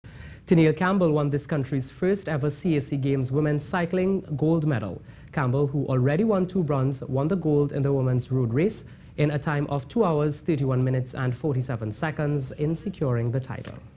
Teniel Campbell Wins Gold At CAC Games
Teniel Campbell won this country's first ever CAC Games women's cycling gold medal.

Campbell, who already won two bronze medals, won gold in the women's road race in a time of two hours 31 minutes and 47 seconds in securing the title.